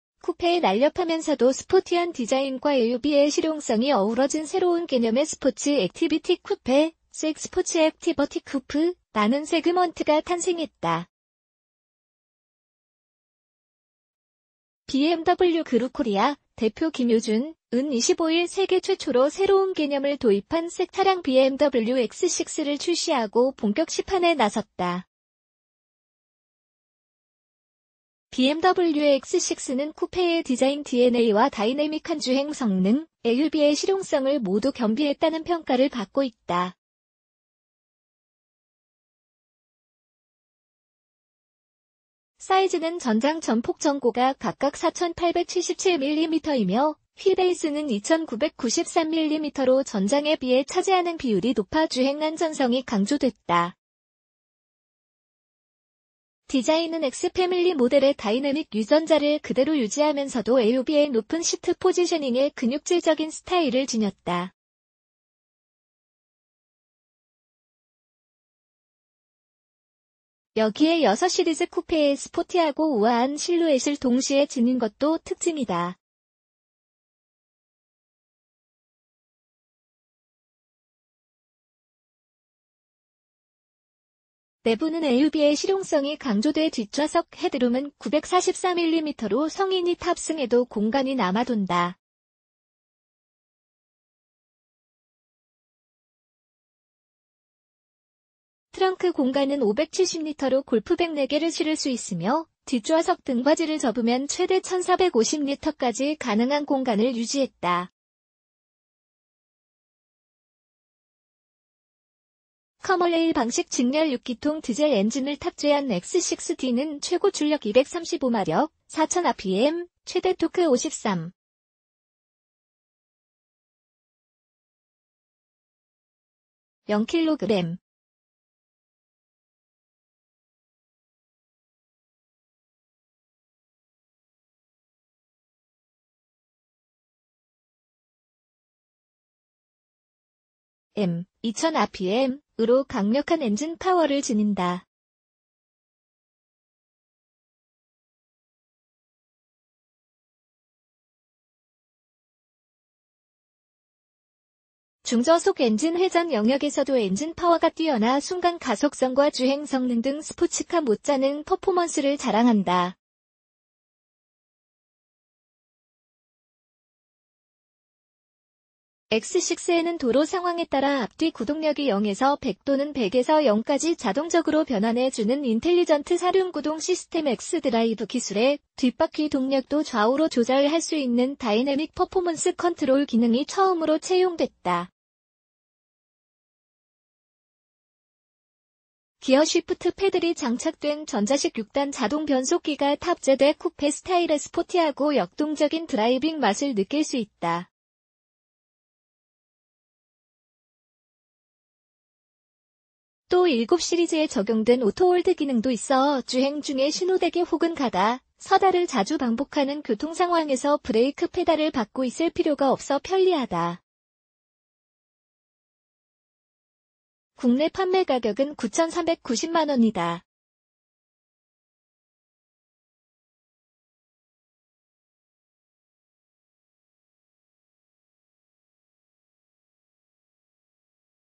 신개념 SAC, BMW X6 출시
\"쿠페의 날렵하면서도 스포티한 디자인과 SUV의 실용성이 어우러진 새로운 개념의 스포츠 액티비티 쿠페(SAC Sports Activity Coupe)라는 세그먼트가 탄생했다.\"

BMW그룹코리아(대표 김효준)은 25일 세계 최초로 새로운 개념을 도입한 SAC 차량 BMW X6를 출시하고 본격 시판에 나섰다.

BMW X6는 쿠페의 디자인 DNA와 다이내믹한 주행성능, SUV의 실용성을 모두 겸비했다는 평가를 받고 있다.

사이즈는 전장*전폭*전고가 각각 4877*1983*1690mm이며, 휠베이스는 2993mm로 전장에 비해 차지하는 비율이 높아 주행안전성이 강조됐다.

디자인은 X패밀리 모델의 다이내믹 유전자를 그대로 유지하면서도 SUV의 높은 시트 포지셔닝에 근육질적인 스타일을 지녔다. 여기에 6시리즈 쿠페의 스포티하고 우아한 실루엣을 동시에 지닌 것도 특징이다.

내부는 SUV의 실용성이 강조돼 뒷좌석 헤드룸은 944mm로 성인이 탑승해도 공간이 남아돈다. 트렁크 공간은 570리터로 골프백 4개를 실을 수 있으며, 뒷좌석 등받이를 접으면 최대 1450리터까지 가능한 공간을 유지했다.

커먼레일 방식 직렬 6기통 디젤 엔진을 탑재한 X6 xDrive30d는 최고출력 235마력(4000rpm) 최대토크 53.0kg.m(2000~2750rpm)으로 강력한 엔진 파워를 지닌다. 중저속 엔진 회전 영역에서도 엔진 파워가 뛰어나 순간 가속성과 주행성능 등 스포츠카 못잖은 퍼포먼스를 자랑한다.

X6에는 도로 상황에 따라 앞뒤 구동력이 0~100 또는 100~0까지 자동적으로 변환해 주는 인텔리전트 사륜구동 시스템 xDrive 기술에, 뒷바퀴 동력도 좌우로 조절 할 수 있는 다이내믹 퍼포먼스 컨트롤 기능이 처음으로 채용됐다.

기어 쉬프트 패들이 장착된 전자식 6단 자동 변속기가 탑재돼 쿠페스타일의 스포티하고 역동적인 드라이빙 맛을 느낄 수 있다.

또 7시리즈에 적용된 오토홀드 기능도 있어 주행 중에 신호대기 혹은 가다, 서다를 자주 반복하는 교통상황에서 브레이크 패달을 밟고 있을 필요가 없어 편리하다.

국내 판매 가격은 9390만원이다.